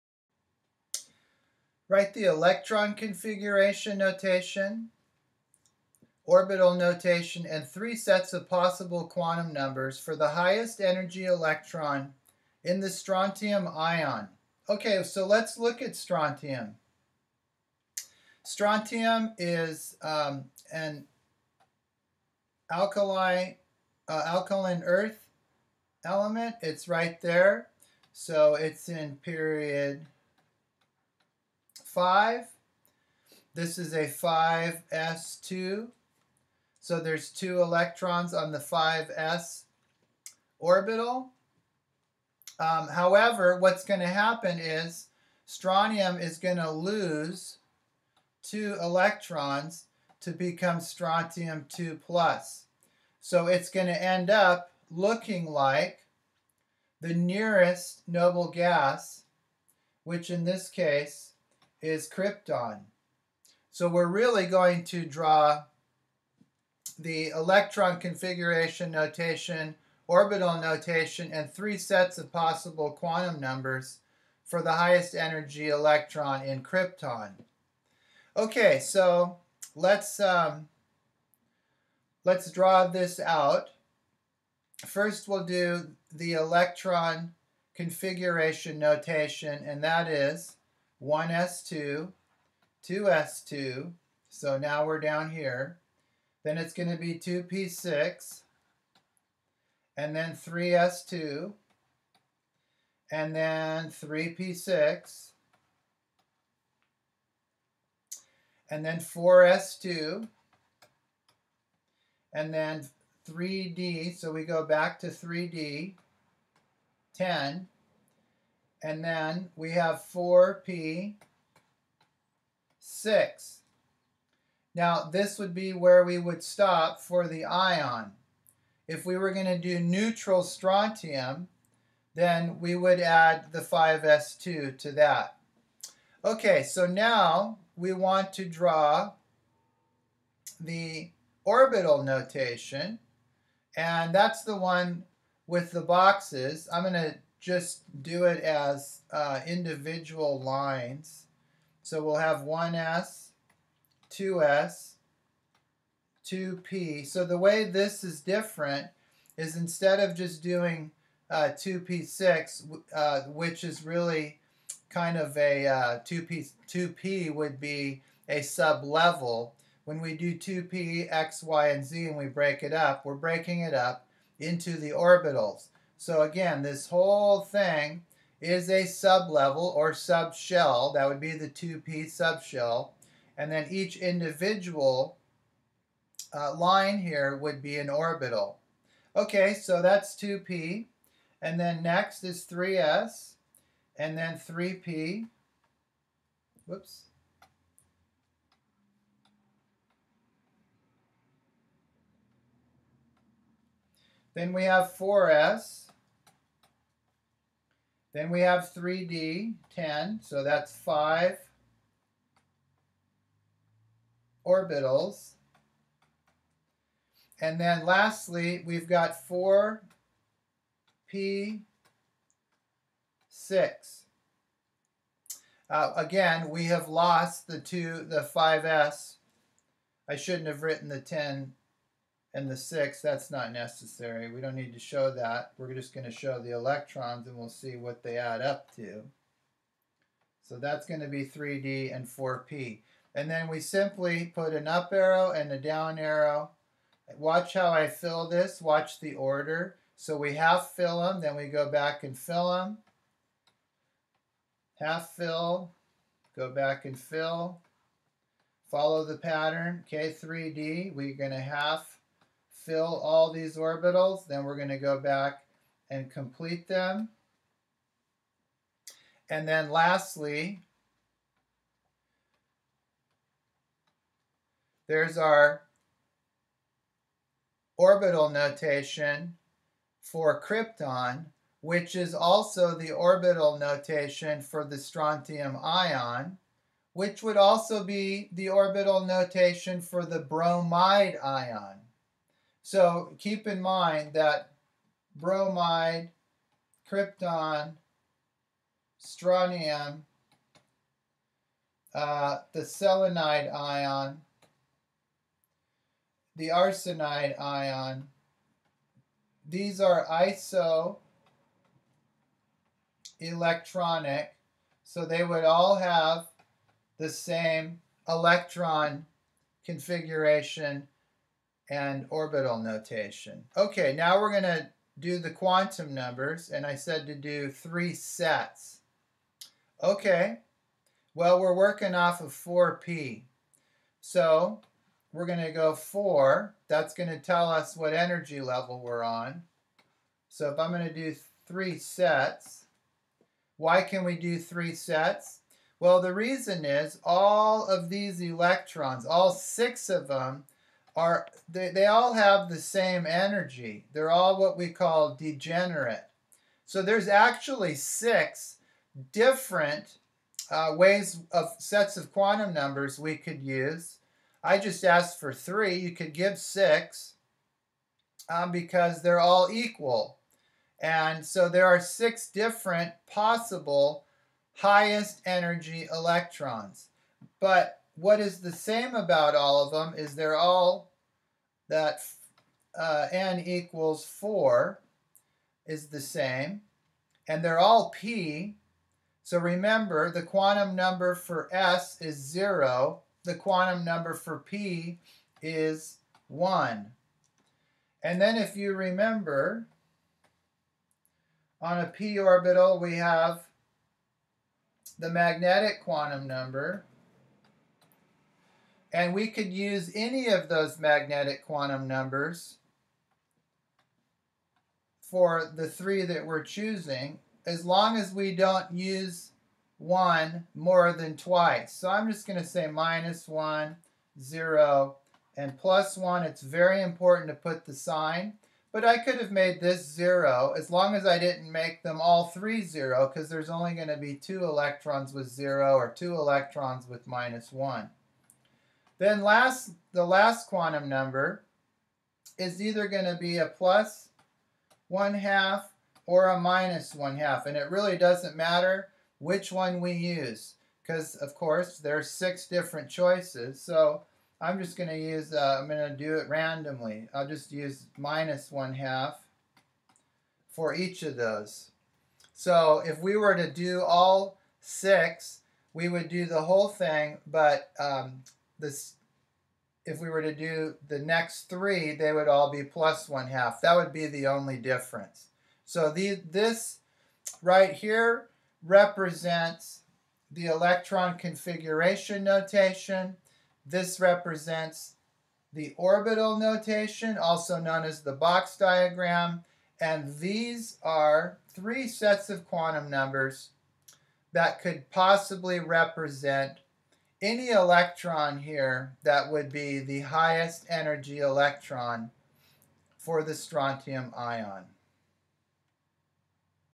Quantum s, orbital diagram and configuration notation for Strontium ion
Draw the electron configuration notation, orbital notation and 3 possible sets of quantum numbers for the highest energy electron of a strontium ion